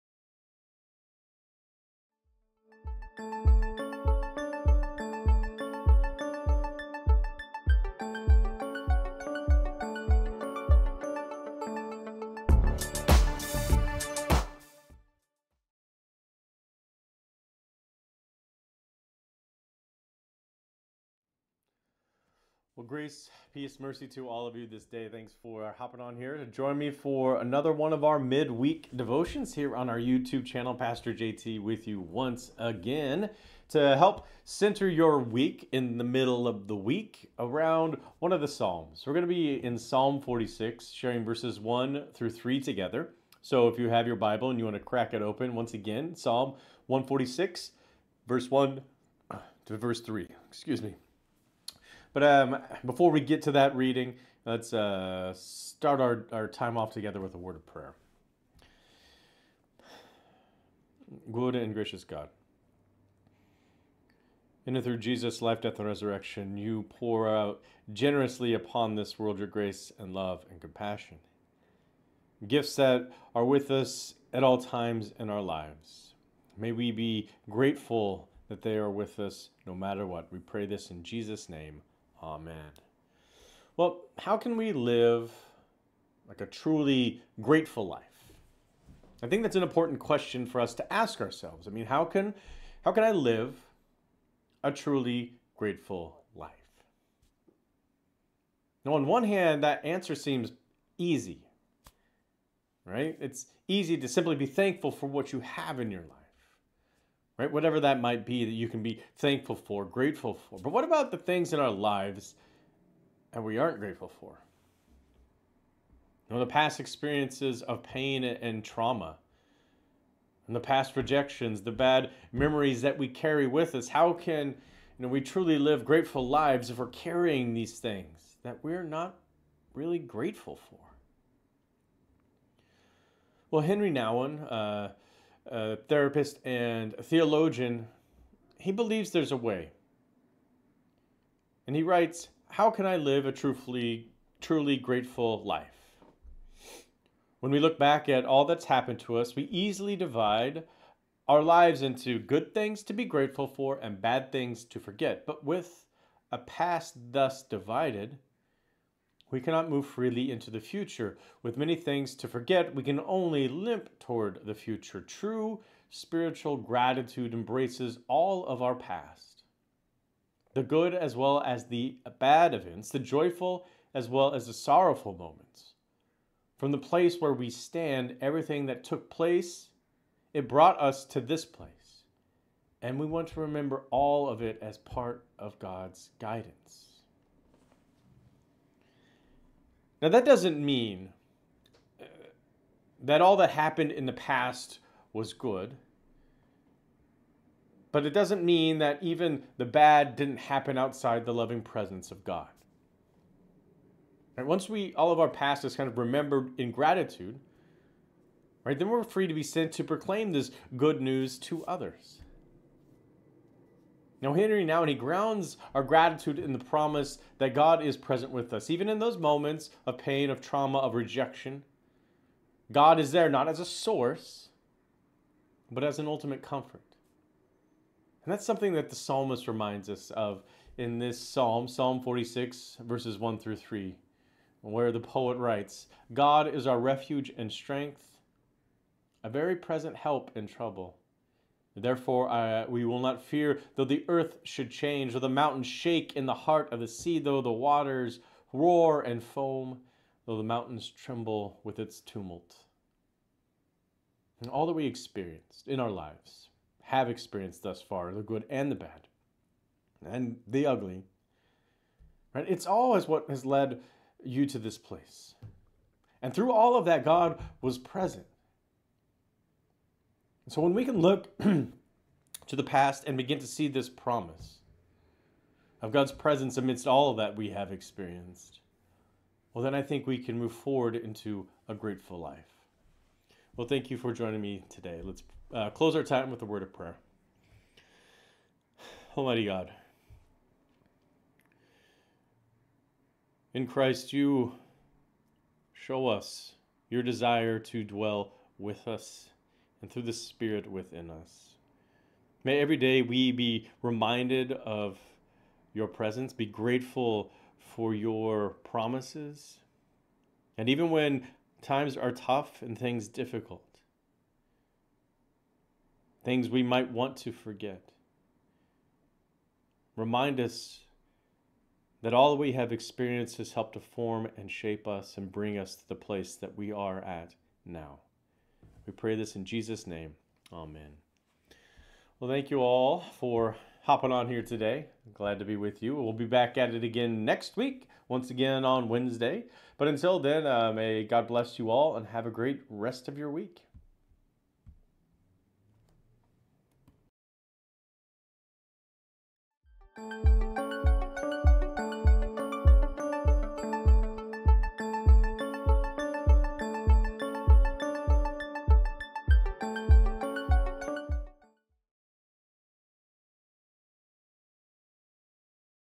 Midweek Devotion 10-16------- A grateful life includes everything.
Thank you for joining me for another midweek devotion. This week we will focus in on Psalm 46:1-3